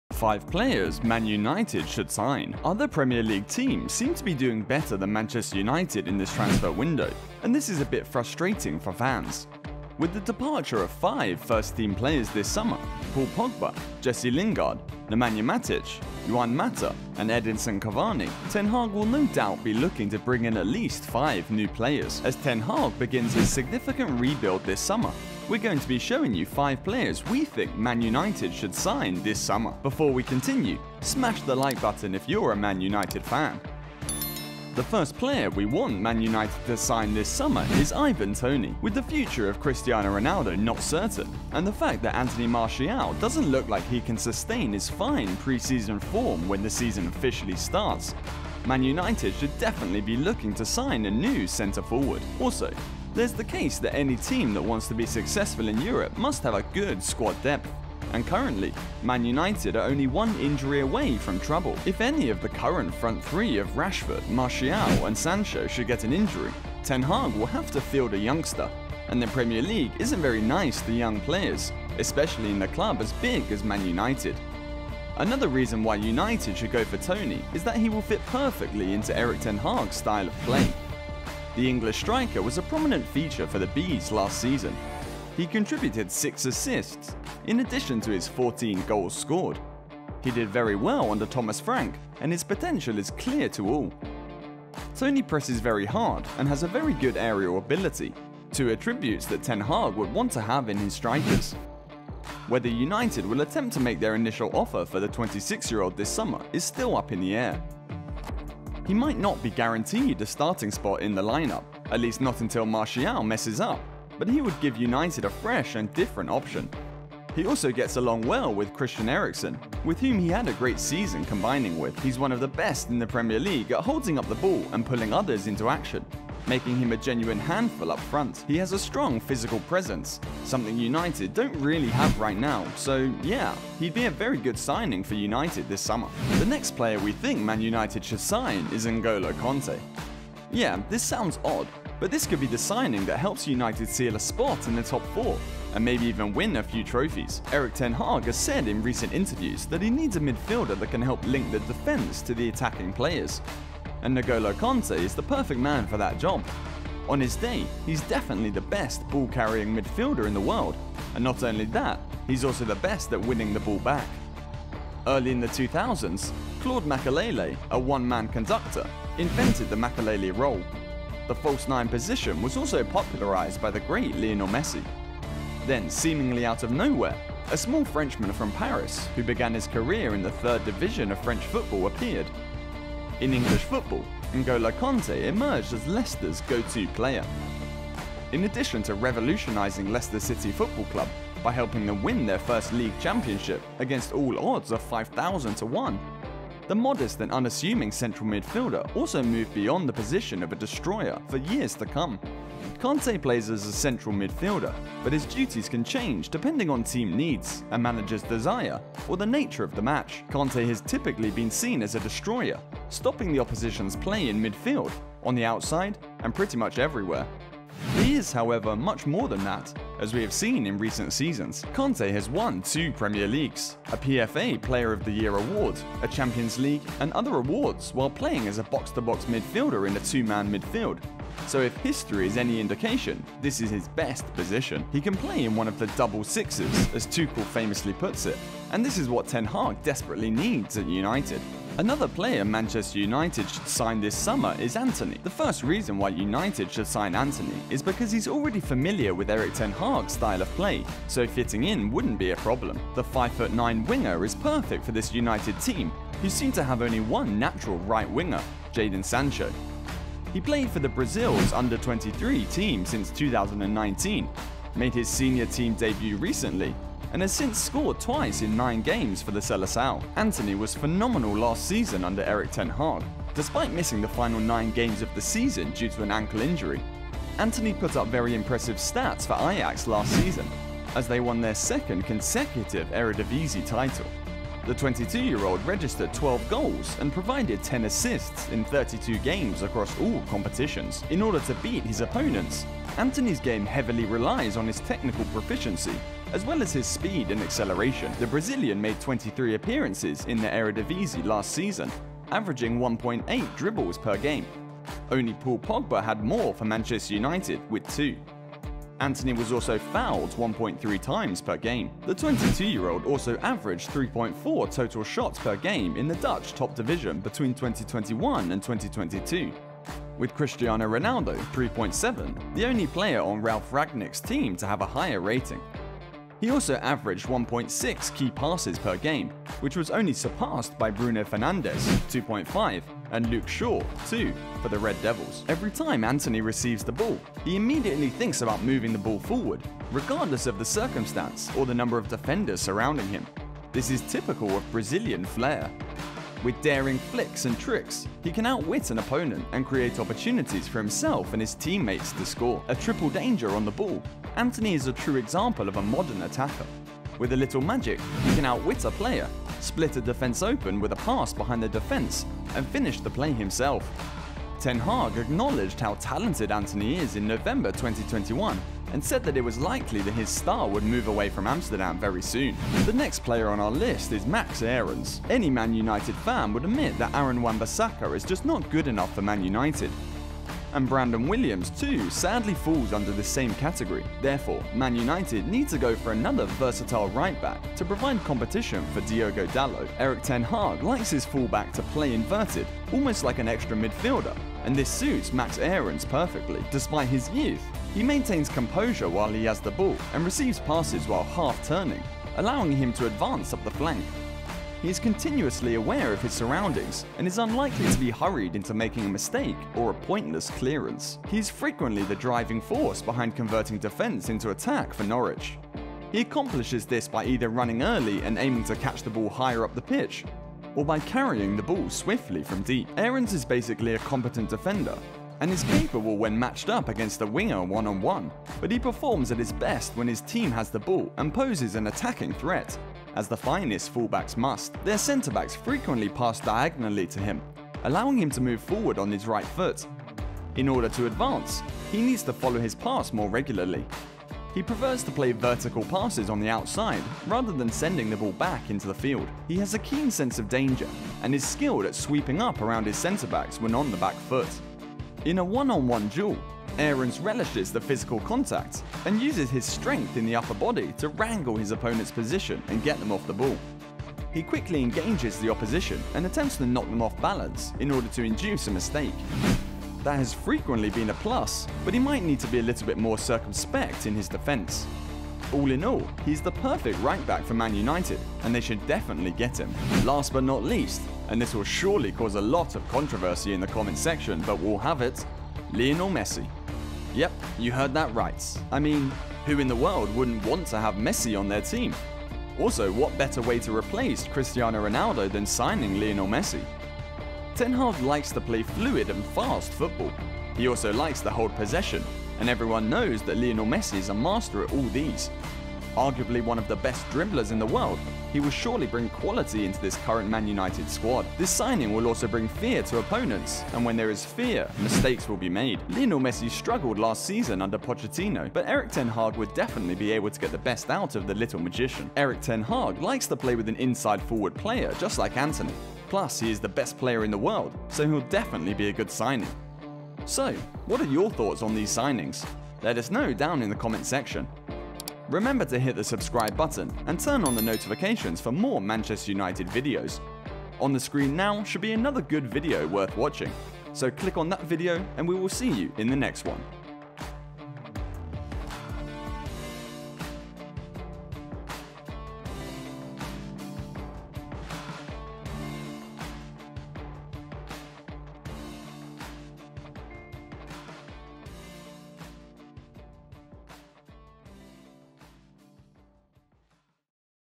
MESSI To Man United? | +5 MORE Man United Signings In Coming?
Other Premier League teams seem to be doing better than Manchester United in this transfer window, and this is a bit frustrating for fans.

With the departure of five first-team players this summer—Paul Pogba, Jesse Lingard, Nemanja Matic, Juan Mata, and Edinson Cavani—Ten Hag will no doubt be looking to bring in at least 5 new players.
As ten Hag begins his significant rebuild this summer, we’re going to be showing you five players we think Man United should sign this summer.

The first player Man United to sign this summer is Ivan Toney.
With the future of Cristiano Ronaldo not certain, and the fact that Antony Martial doesn't look like he can sustain his fine pre-season form when the season starts officially, Man United should definitely be looking to sign a new center forward.

Also, there’s also the case that any team that wants to be successful in Europe must have a good squad depth, and currently, Man United are only one injury away from trouble. If any of the current front three of  Rasford, Martial, and Sancho should get an injury, Ten Hag will have to field a youngster, and the premier league isn’t very nice to young players, especially in a club as big as Man United.